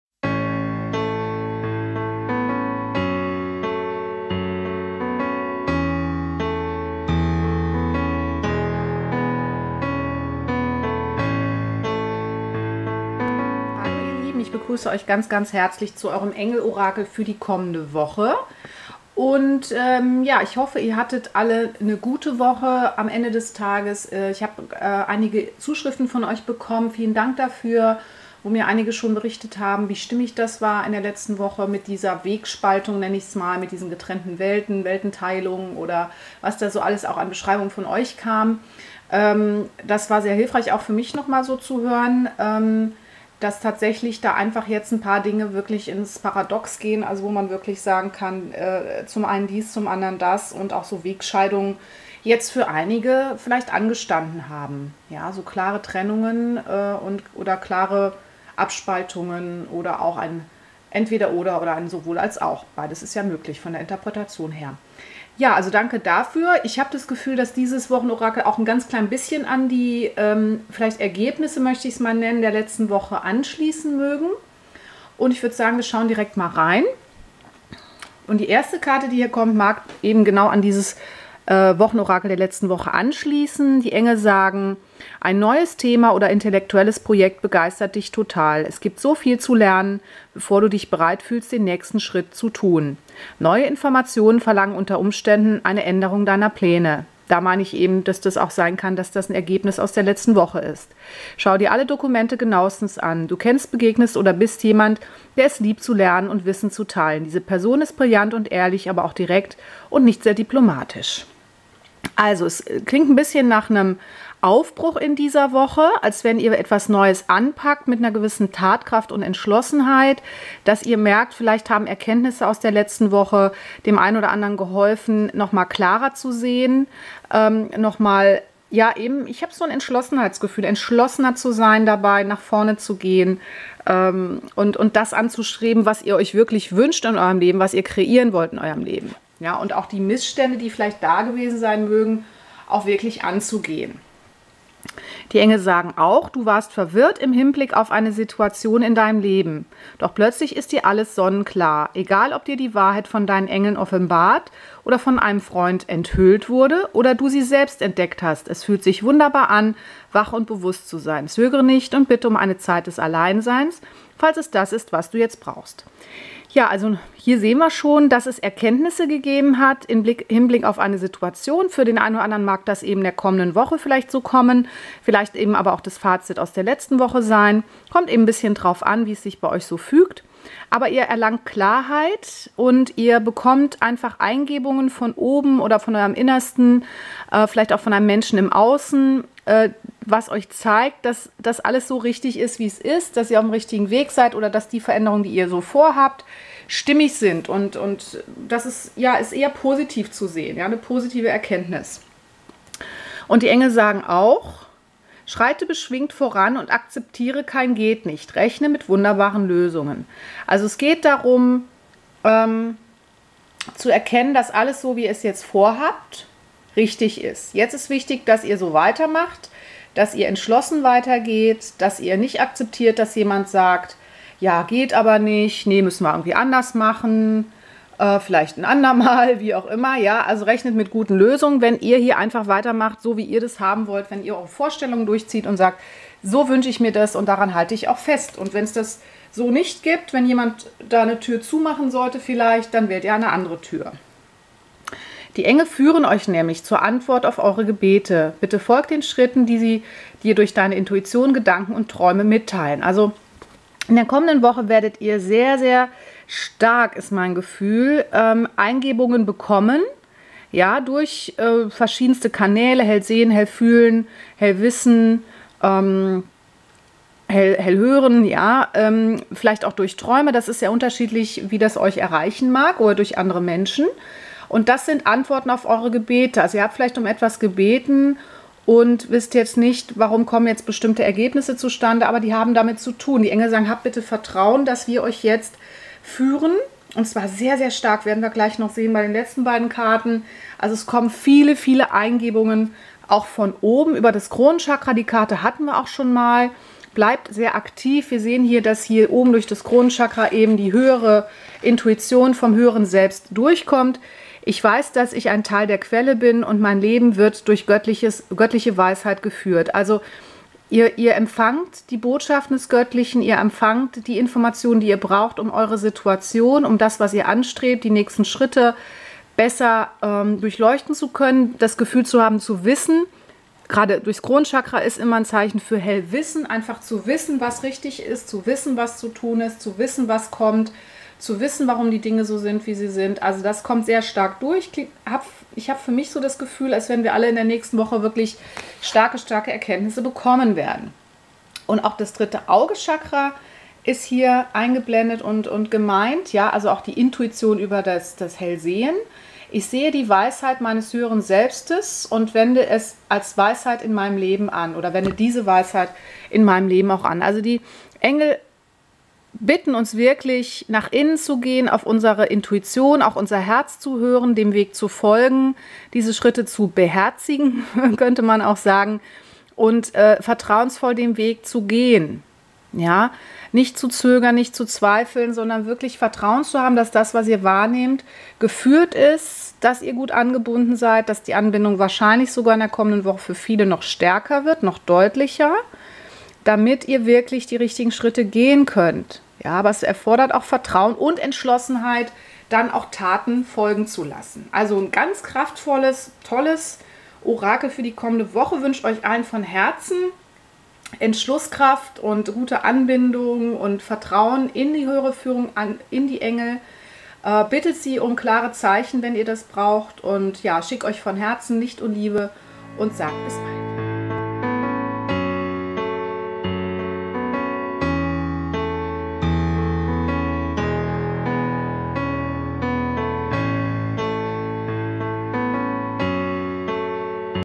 Engel Orakel der Woche vom 08. bis 14. Juni 2015
// Verstärkte Führung durch die Engel und dein Höheres Selbst
In der kommenden Woche gehst du gestärkt aus den möglicherweise geschehenen Konflikten der letzten Woche in neue Projekte und Vorhaben hinein, deren Wichtigkeit und Bedeutung für deine höheren Ziele dir klar geworden ist. Die Engel signalisieren deutlich, dass du jetzt selbstbewusst voran schreiten kannst, denn "geht nicht, gibt's nicht". Verlasse dich daher voll und ganz auf deine innere Führung, durch die die Engel und dein Höheres Selbst dir gerade in der kommenden Woche verstärkt und zuverlässig ihre Führung zuteil werden lassen…